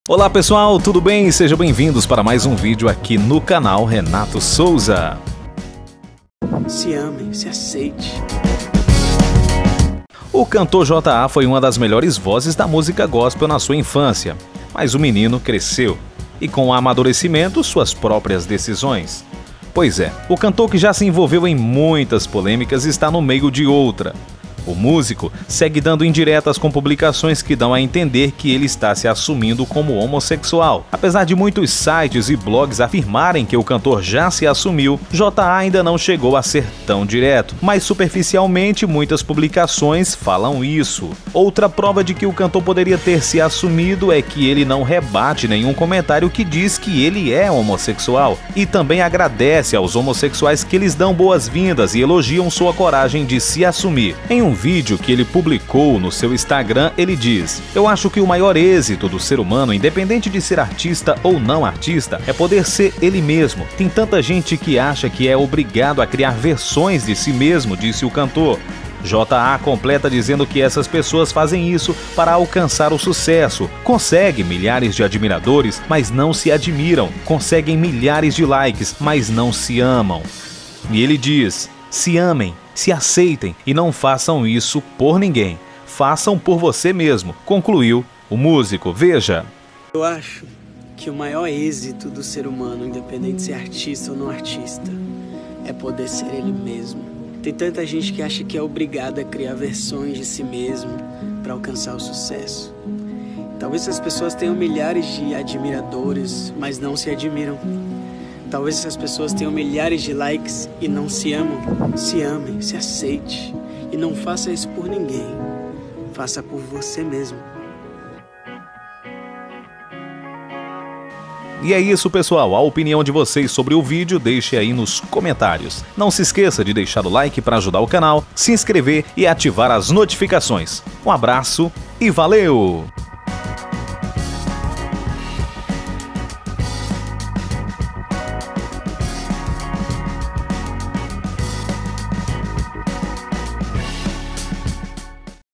PUBLICAÇÕES DE JOTTA A CAUSA POLÊMICA, SE ASSUMIU?
O cantor gospel Jotta A, permanece confundindo a cabeça dos seus seguidores. Após supostamente sair do  “armário” na última semana, o músico segue dando indiretas com publicações que dão a entender que ele está se assumindo.

Deixe seu compartilhe a sua opinião sobre o assunto do video
deixe nos comentarios, Obrigado.